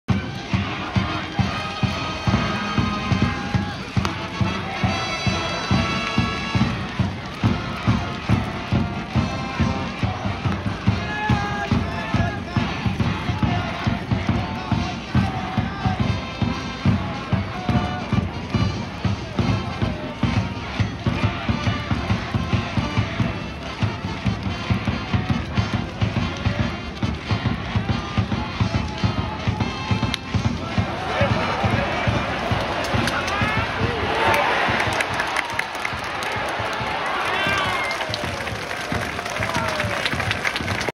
慶應義塾大学、渡部遼人の内野安打
2021/10/31
早稲田大学対慶應義塾大学
慶應義塾大学、渡部遼人ショート内野安打を打つ。
慶應義塾大学1点。